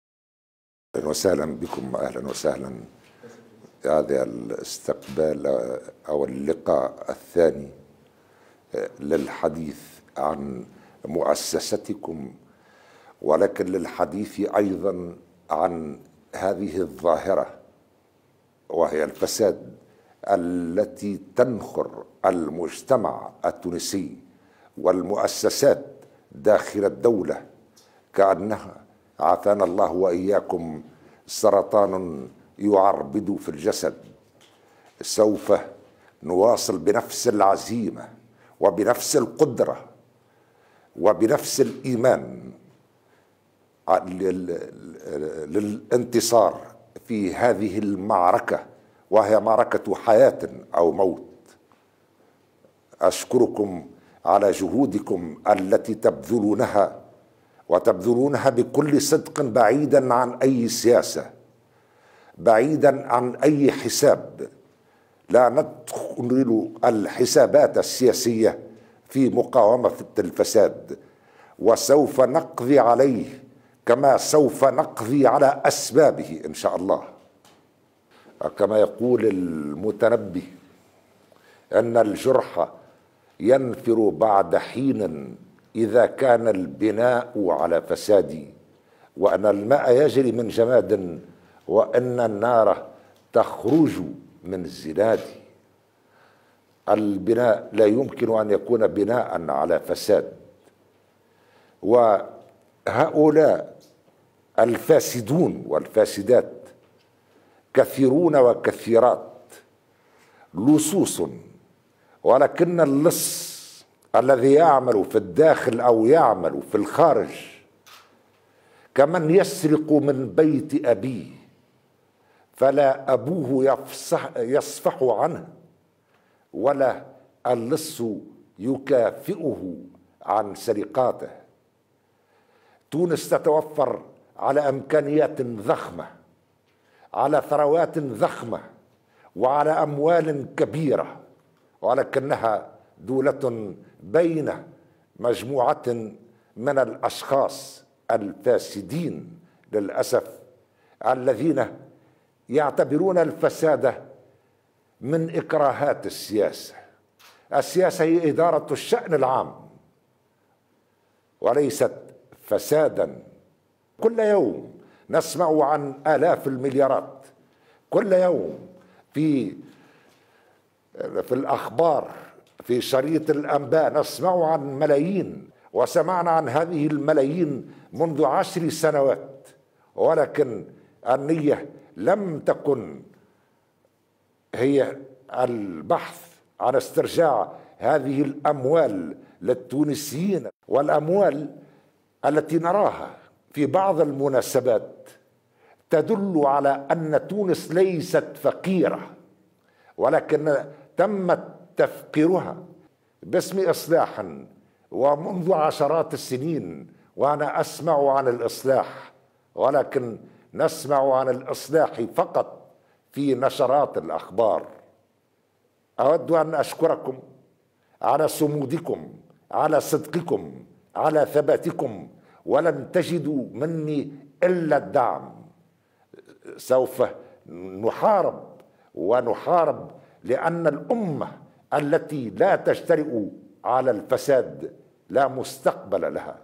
قيس سعيد : الأموال التي نراها في بعض المناسبات تدل على أن تونس ليست فقيرة و لكن تم تفقيرها
استقبل رئيس الجمهورية قيس سعيد اليوم الأربعاء 3 مارس 2021 بقصر قرطاج رئيس الهيئة الوطنية لمكافحة الفساد السيد عماد بوخريص.

وتم التطرق خلال اللقاء إلى أنشطة الهيئة في المدة الأخيرة والتأكيد على ضرورة العمل لوقف ظاهرة الفساد التي تزداد استفحالا وتخرّب الدولة والمجتمع على السواء.

وقد تم التعرض في هذا الإطار بالخصوص إلى الأوضاع التي تعرفها عديد المؤسسات والمنشآت العمومية، وإلى وجود فاسدين يريدون الاستفادة من المال العام وفاسدين مفسدين يستفيدون من ثروات الشعب التونسي ويهيئون بعد ذلك السبل للتفريط فيها.